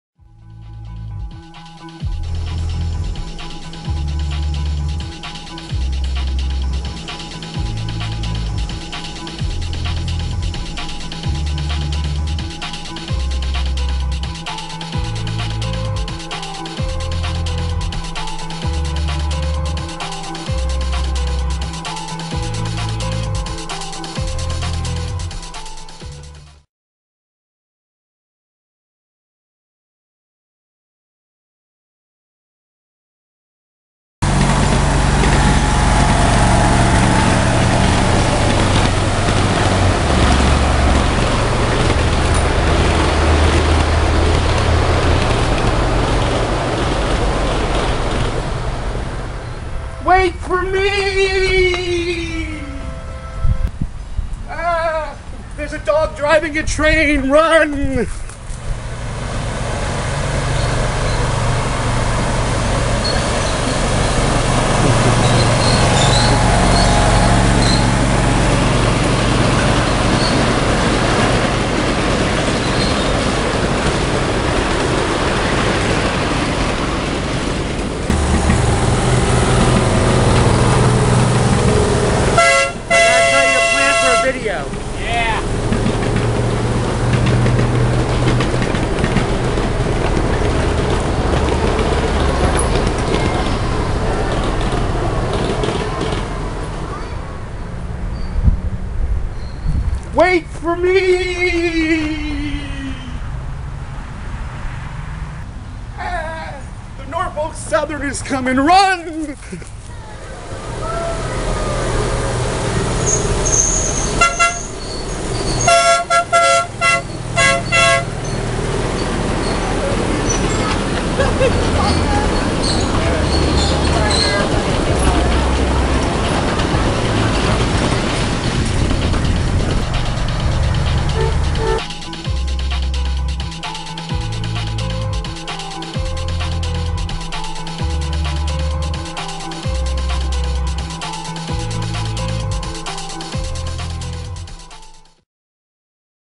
Seaboard System Dog On Train Runs Man Out Of Tunnel
This is a video I made of the Largo Central Park Railroad in Largo Florida where a dog on a Seaboard locomotive chases me out of a train tunnel and much more.